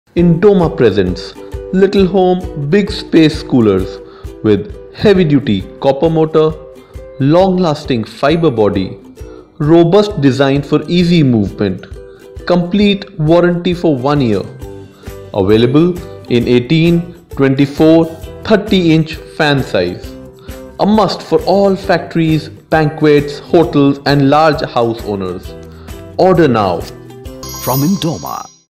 Littelhome Big Space Coolers I For Factories, Hotels , Banquets & Large Houses
Whether you are a factory, banquet, hotel/club or large house owner the need for cooling outdoor area / halls is a must in scorching summer. Get the ease of portable cooling now with Littelhome Big Space Coolers.

Feature:-
1. Heavy Duty Copper wire
2. Long-Lasting Fibre Body
3. Robust Design
4. One Year Warranty
Available in 18”, 24”, 30” fan size.

Indoma Industries P . Ltd.
311, Ecotech 1 Extn.
Greater Noida
UP - 201308